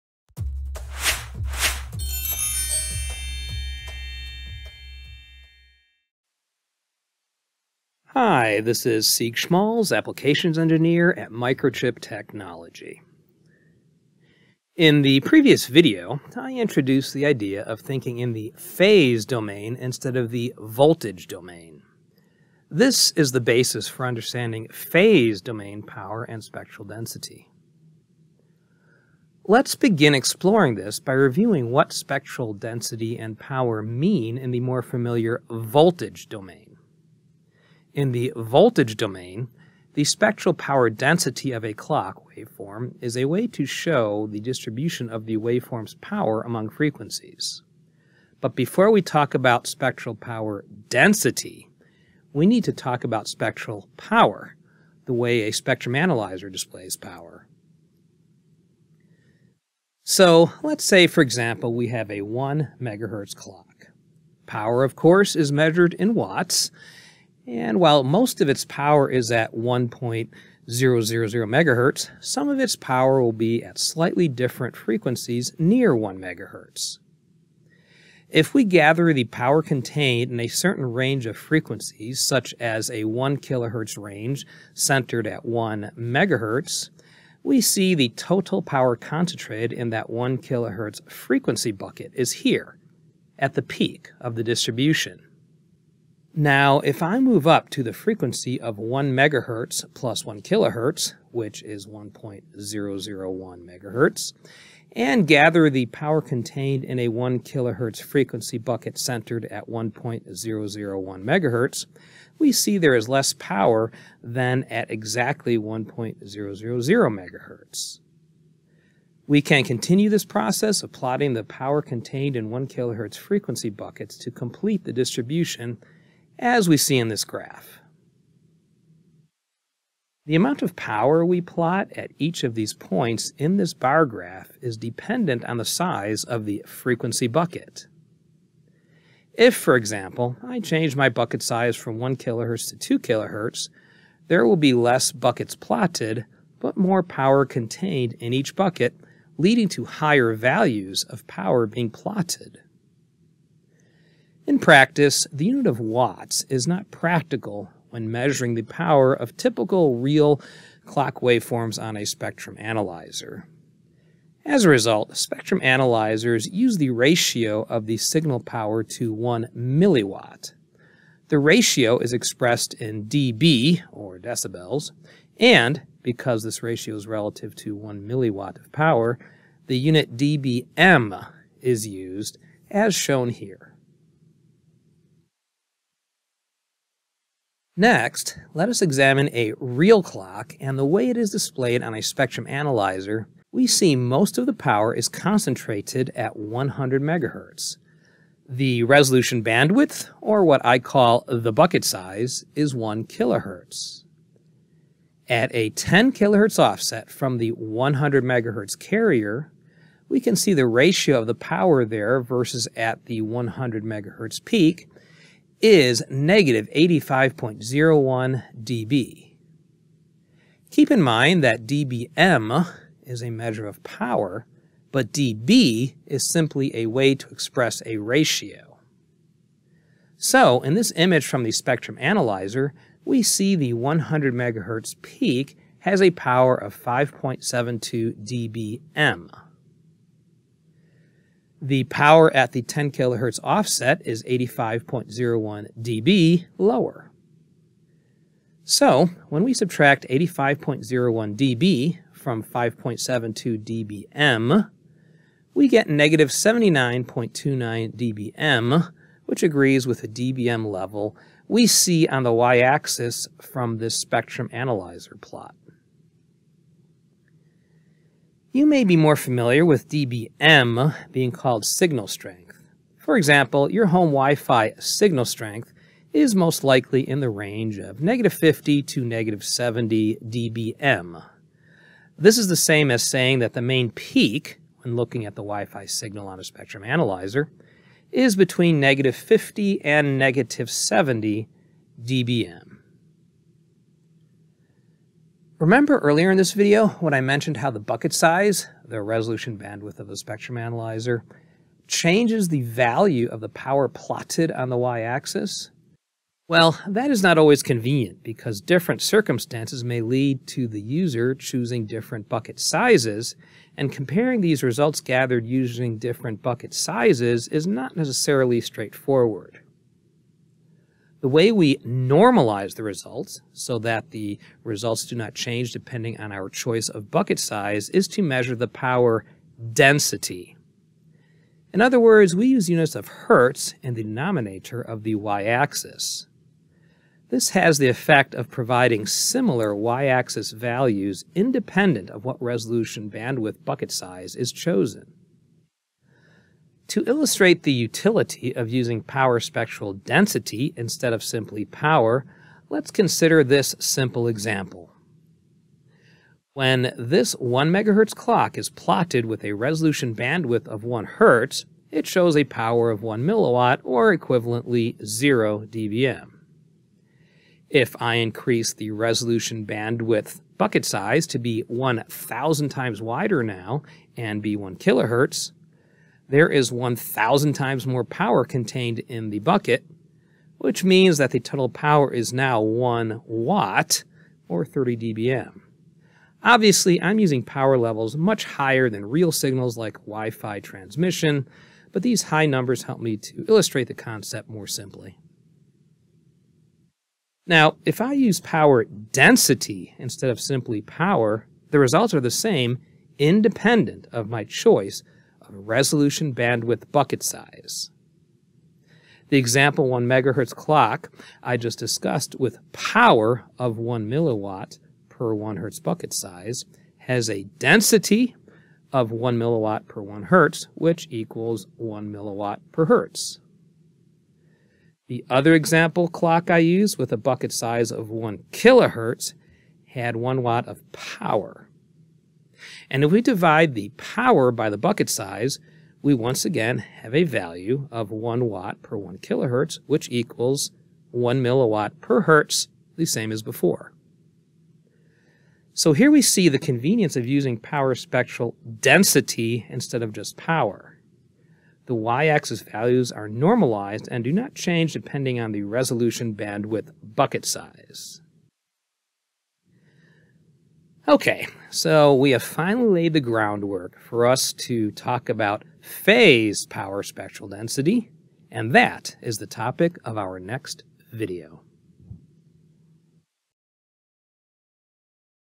Spectral Power Density: A Review
Timing specifications typically include parameters such as "Phase Noise" and "Jitter", which are  related to a clock's Phase Domain Spectral Power Density. This video reviews Voltage Domain Spectral Power and Spectral Power Density, which lay the groundwork for understanding Phase Domain Spectral Power Density.